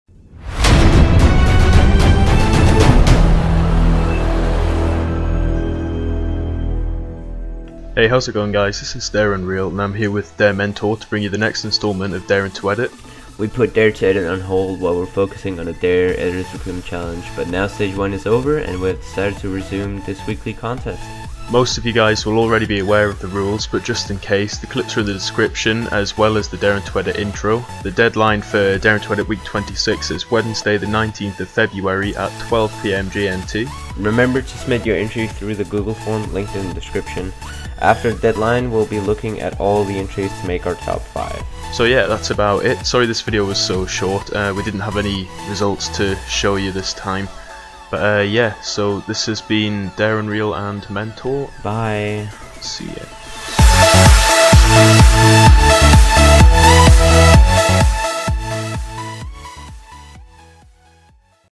Daring to Edit Week 26
WATCH IN 720P! Please remember to leave a like/fav/comment if you enjoyed the video! :) [Click more for Description]

As you know we put Daring to Edit on hold while we were hosting Stage 1 of the Dare Editors Recruitment Challenge. Now that it's over, we are resuming with DTE Week 26! Download link for the clips are below. As usual, your entry must the given clips, and include the DTE Intro. We can't wait to see the see all the entries this week, good luck, and have fun!

Deadline: Wednesday 19th February (12pm GMT)

Dare Artelier Twitter!